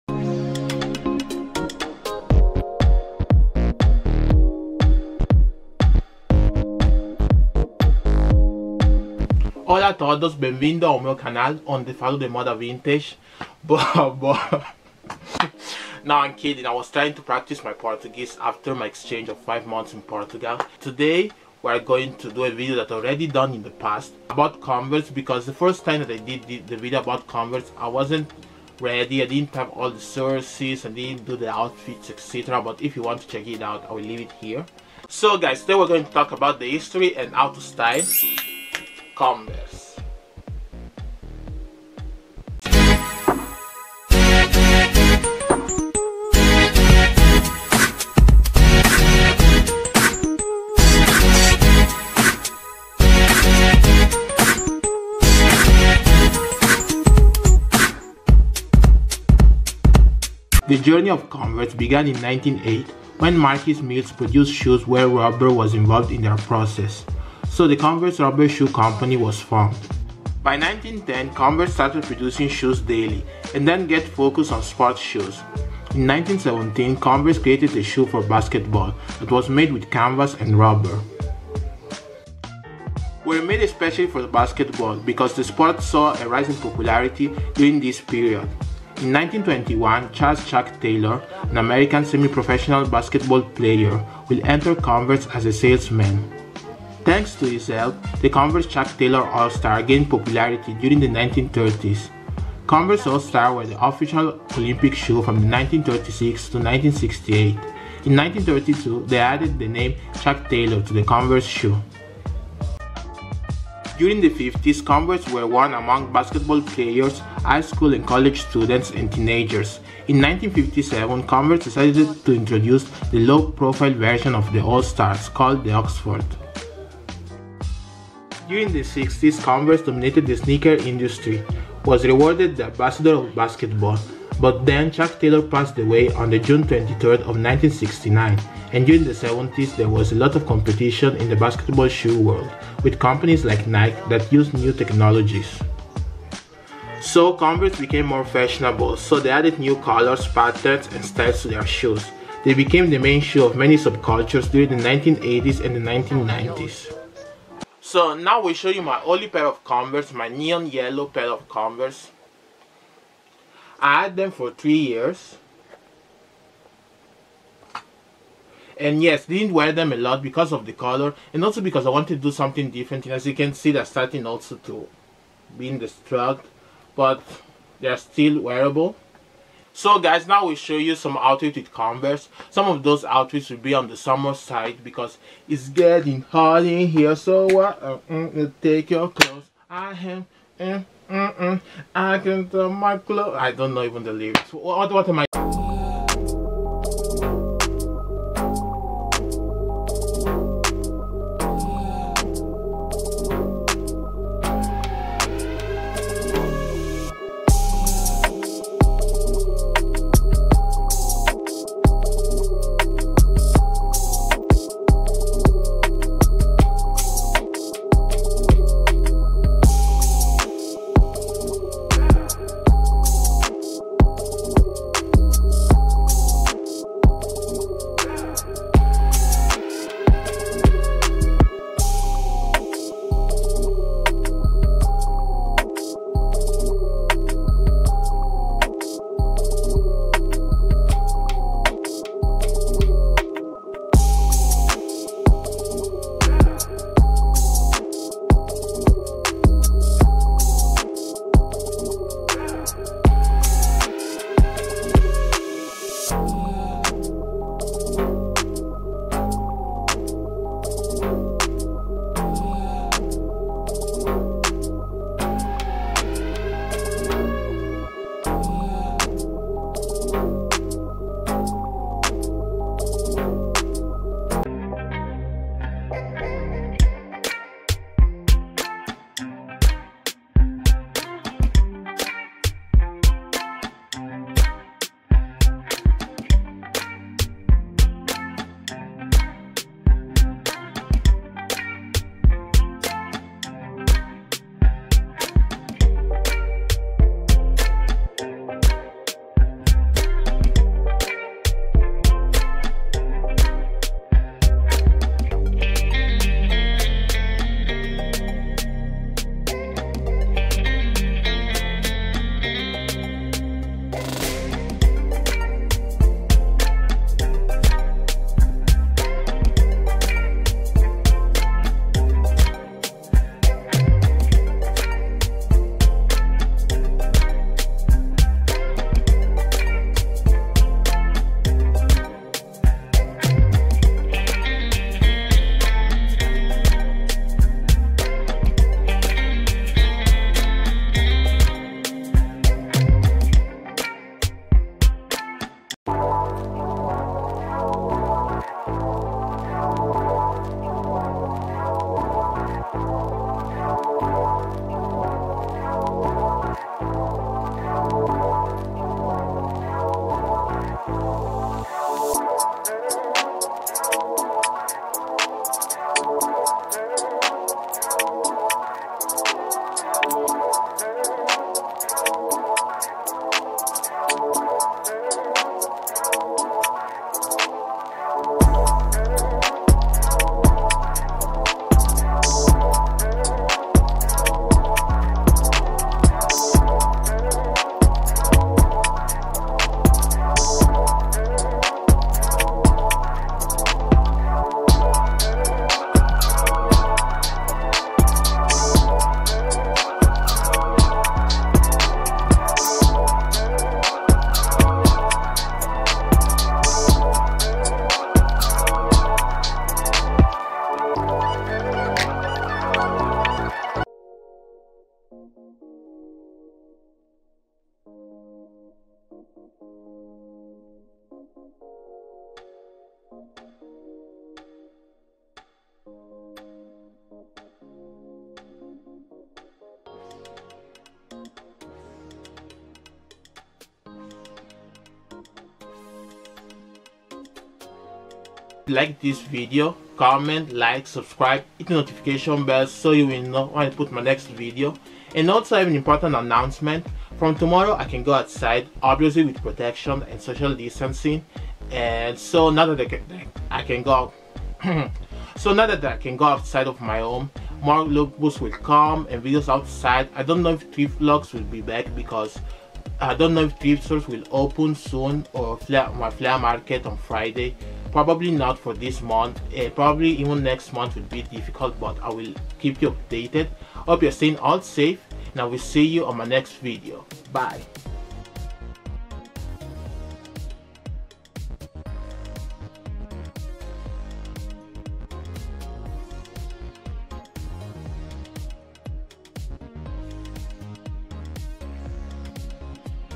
History/How to Style Converse All Star
This is History/How to Style Converse all Star.
In this video i will talk about the history of Converse all Star that became popular thanks to Charles Chuck Taylor and how to style this iconic shoe. I will show you three outfits that i paired with Converse and talk about the journey of Converse from 1908 to the their success during the 50s and 60s and beyond.
I did already a video about Converse in 2017 but wasn`t that great because i did not have all the sources and i did not create some outfits around them so that is why i am doing this video again.